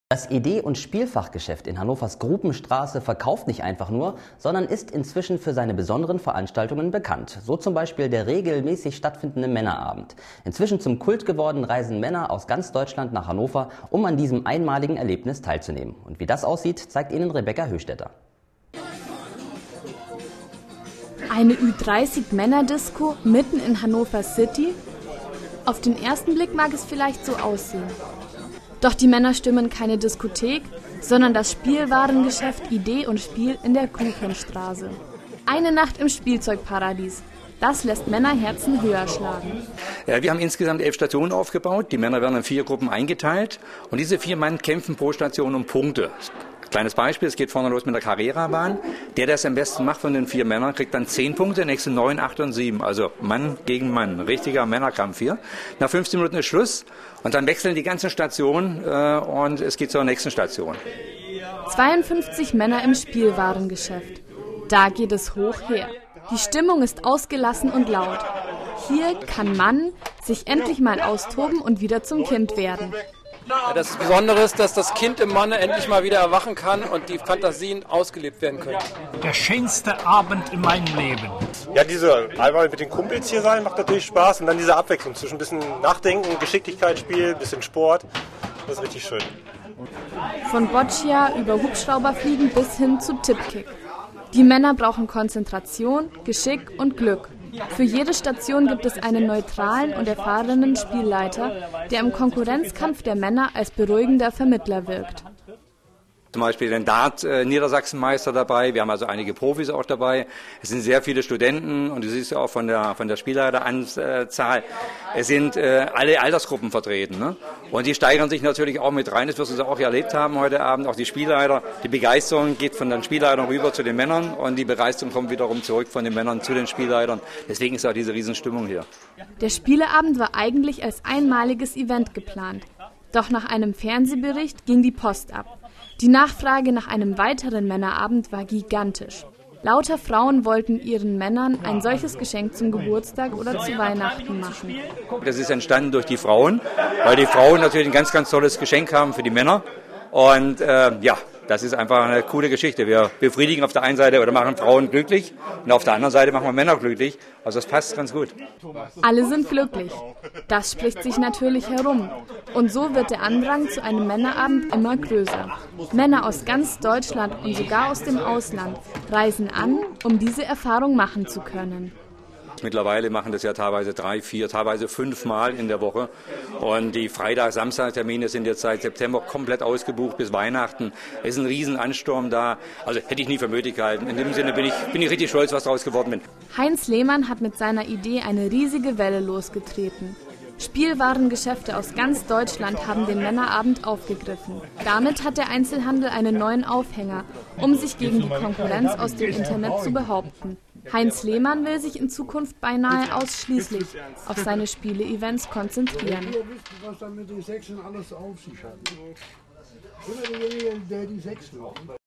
Männer-Spieleabend im "Idee+Spiel"-Fachgeschäft in Hannover
Einmal im Leben in einem Spielwarengeschäft eingesperrt sein.
Davon träumt so ziemlich jeder.
Im Idee+Spiel ist das möglich: Inzwischen drei Mal die Woche können sich Männer in dem Geschäft austoben und elf verschiedene Spiel-Station durchlaufen.
Da wird Man(n) wieder zum Kind.

Kamera und Schnitt: Rebecca Hoechstetter
Redaktion: Vincent Reinke, Rebecca Hoechstetter